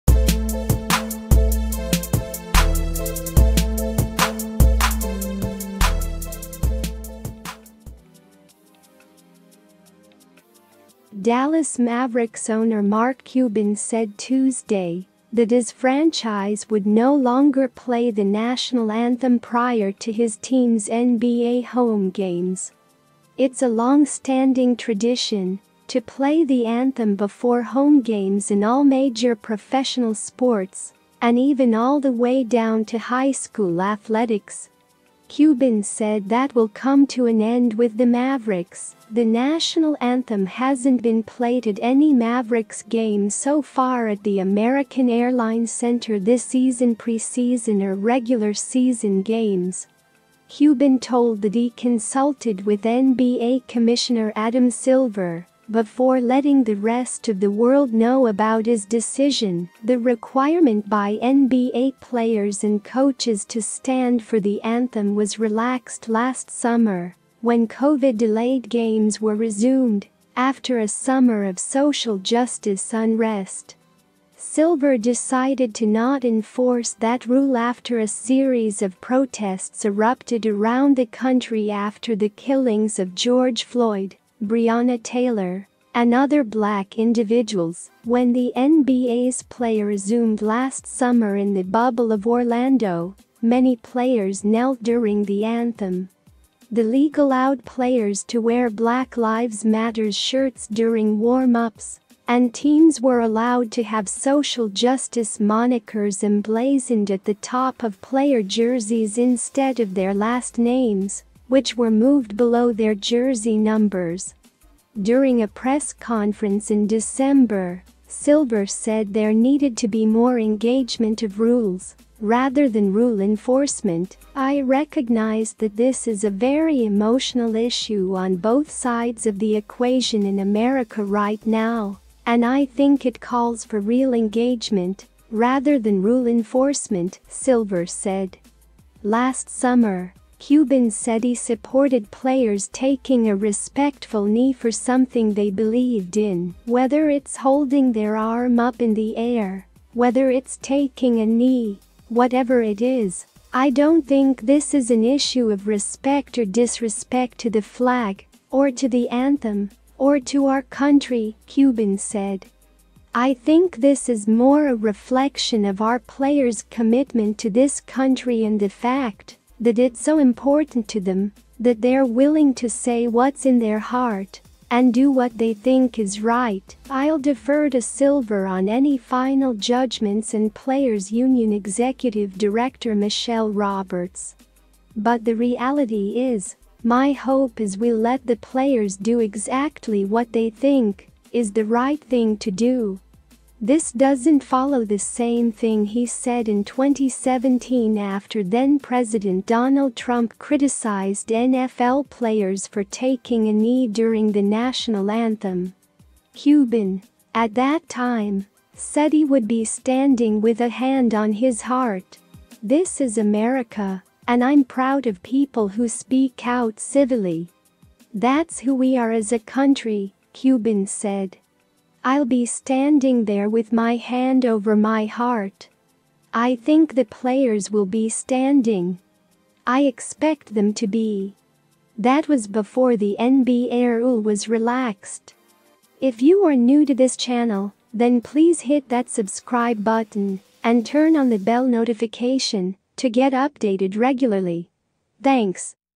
Mark Cuban Confirms The Dallas Mavericks Won't Play National Anthem Anymore
Dallas Mavericks owner Mark Cuban said Tuesday that his franchise would no longer play the national anthem prior to his team's NBA home games. It's a long-standing tradition to play the anthem before home games in all major professional sports, and even all the way down to high school athletics.